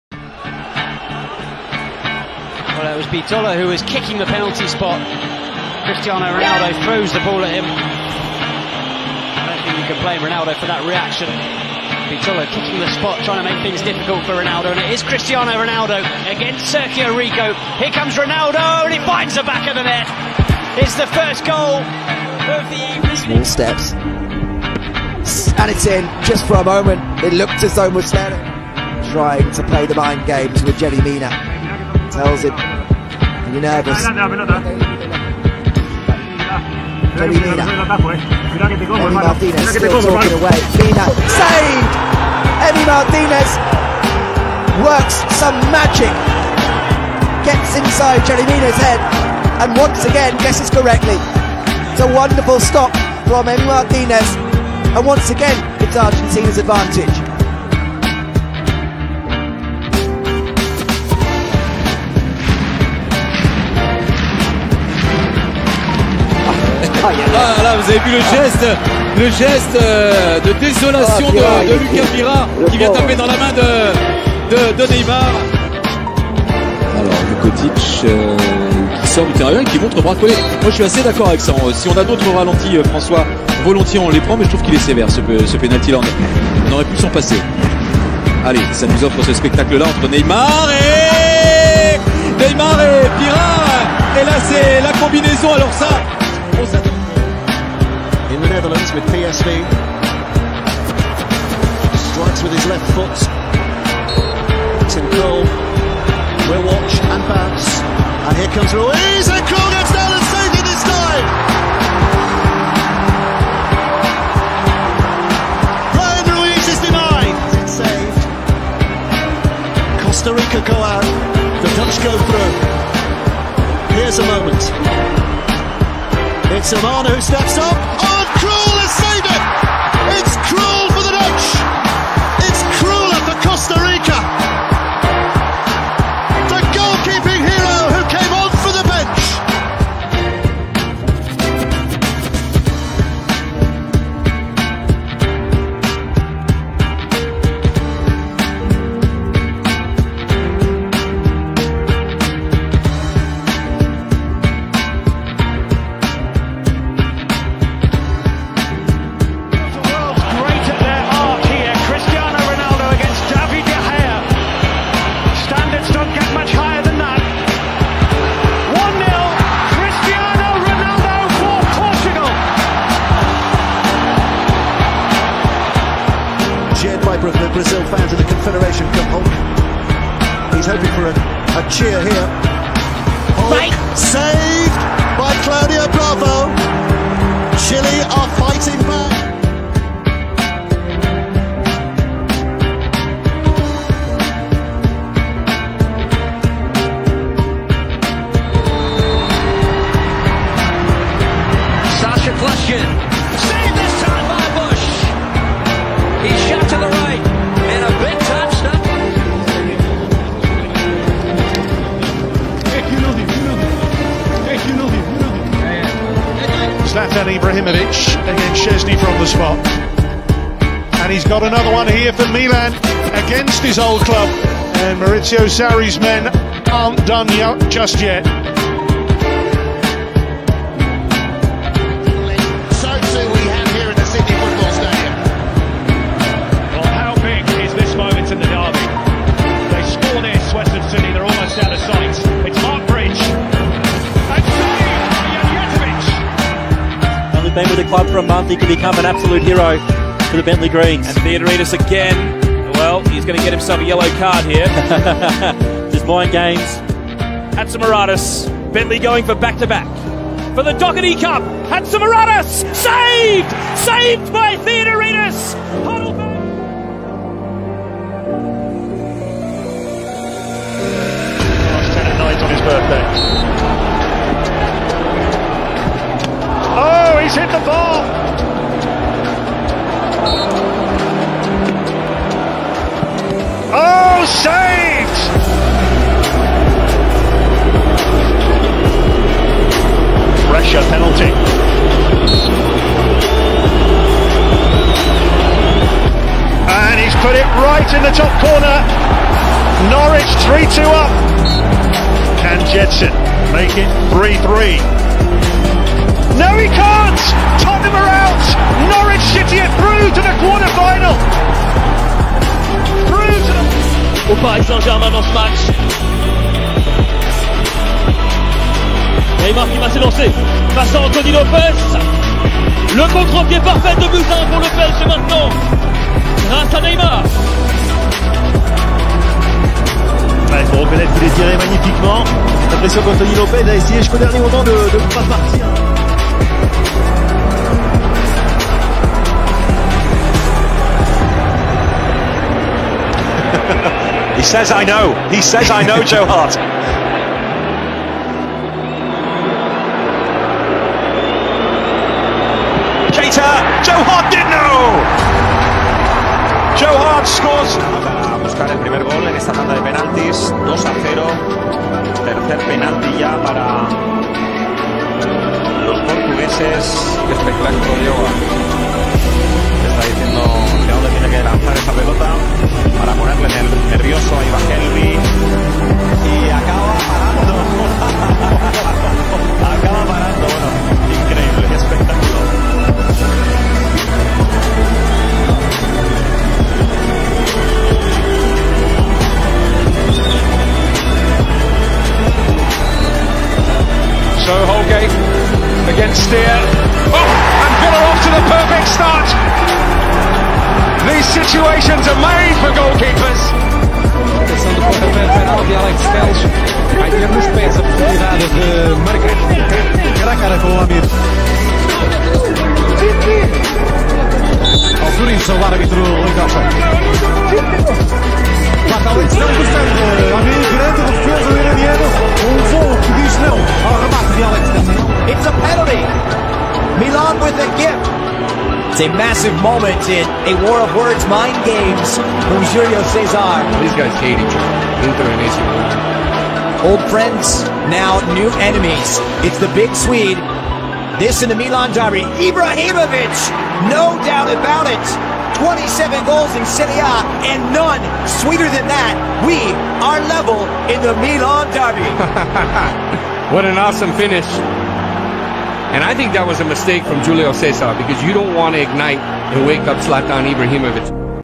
Football Penalty Mind Games Ronaldo, Messi, Neymar | Football Penalty Skills 2021 | Football Tricks
When is a penalty awarded in football ! Football Penalty Mind Games Compilation Ronaldo, Messi, Neymar. Enjoy the high end mind games by the players for amazing strike.